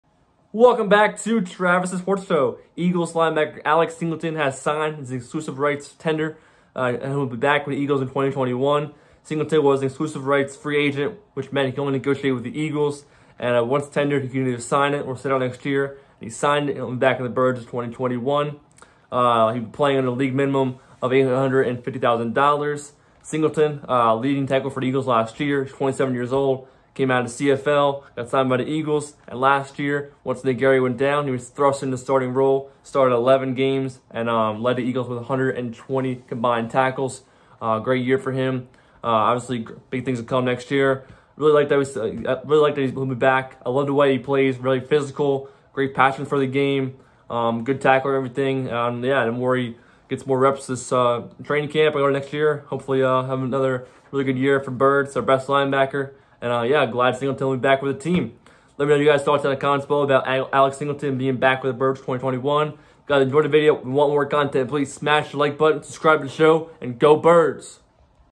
ALEX SINGLETON SIGNS HIS EXCLUSIVE RIGHTS TENDER!! | WILL RETURN TO THE EAGLES!
Eagles linebacker Alex Singleton has signed his exclusive rights tender and will return to the Eagles next season! He was the Eagles leading tackler in 2020! Go Birds!